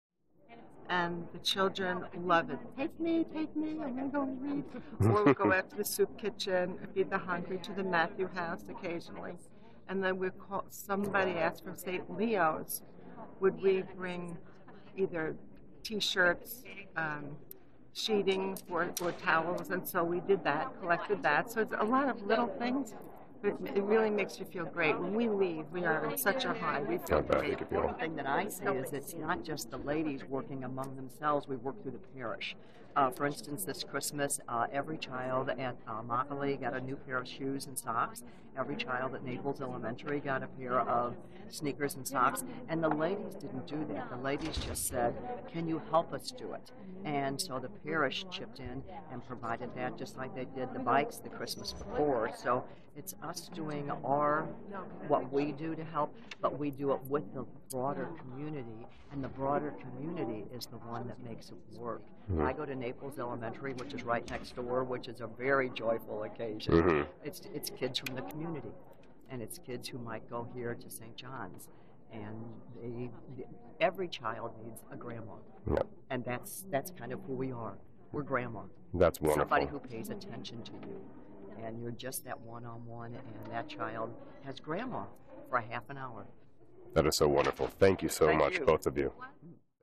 Ladies of Charity 2014 Commissioning Ceremony
The Ladies of Charity explain their role in supporting the community around Saint John during the Ladies of Charity 2014 Commissioning Ceremony